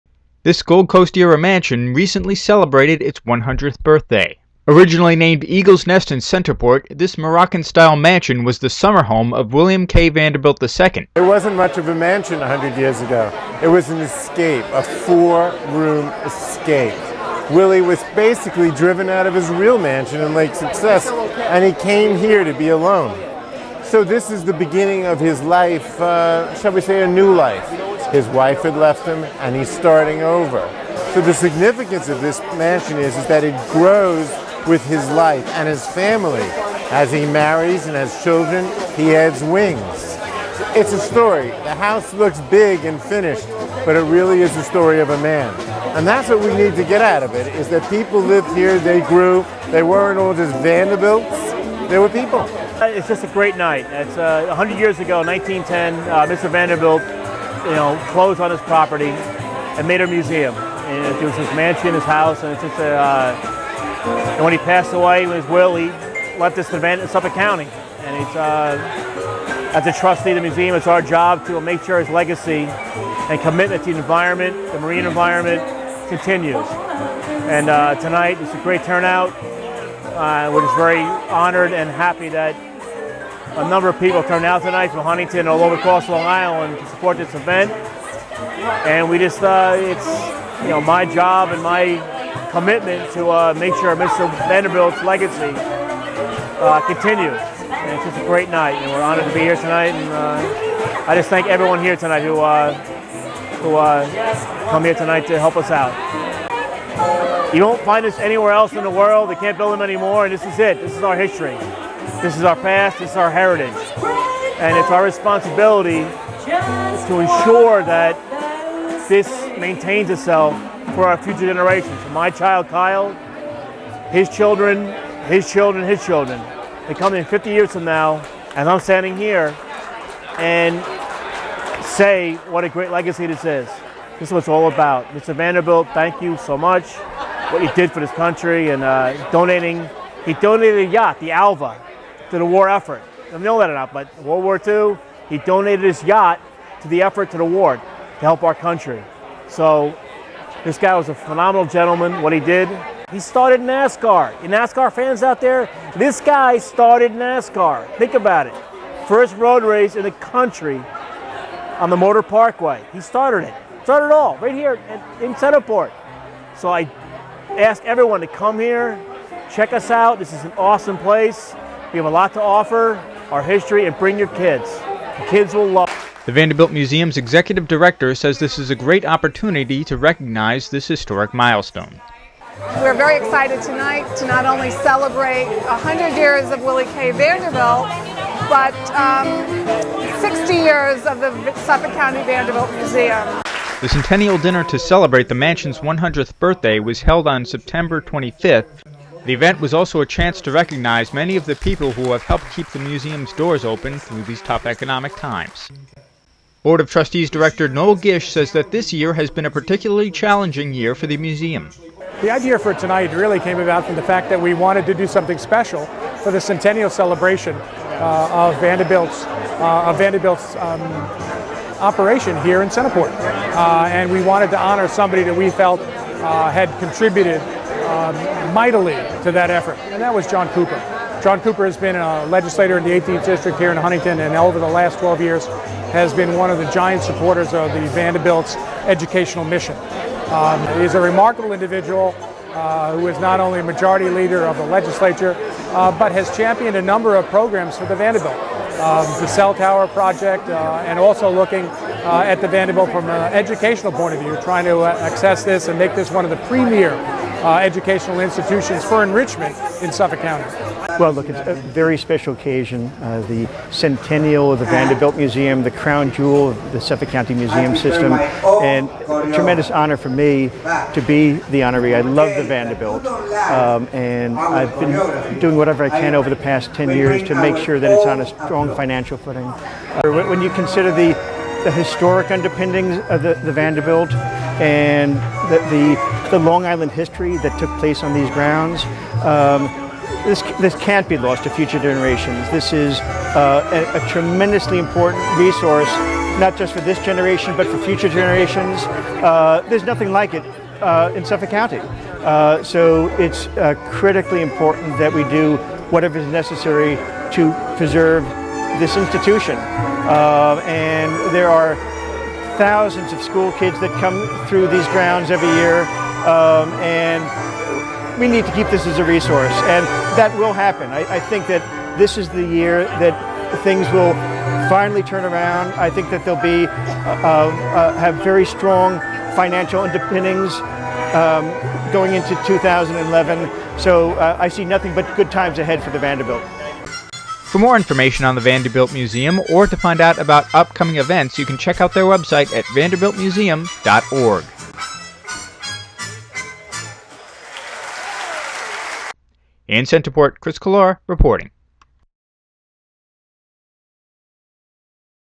Vanderbilt Mansion Turns 100
Chris Collora reports on the 100th Centennial celebration of the Vanderbilt Museum in Centerport. This Gold Coast Era Mansion, named Eagle's Nest, was the summer home of Willaim K. Vanderbilt II.